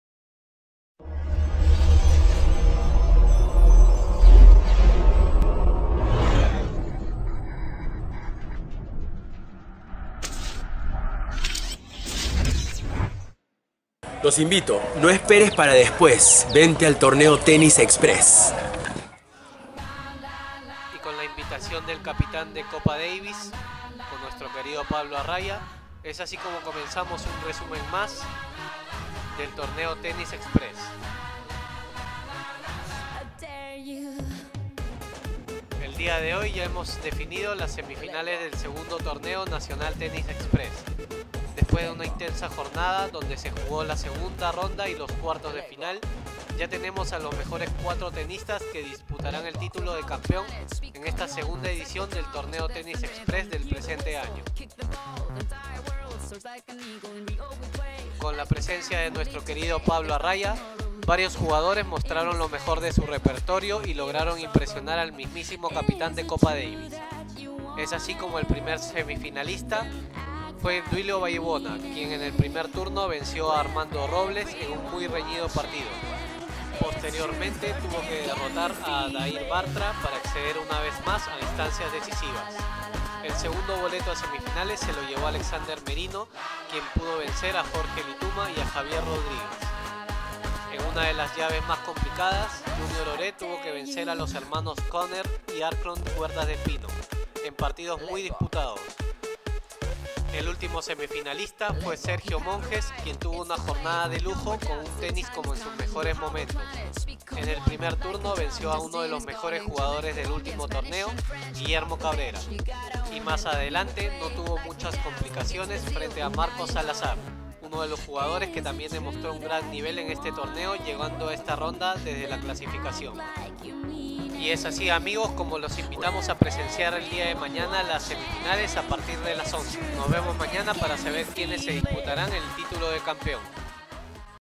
Segundo Torneo Tennis Express - DIA 4
Con la presentación de Pablo Arraya, a continuación te mostramos algunas escenas de los participantes del primer día del CUADRO PRINCIPAL en este II Torneo de Tennis Express.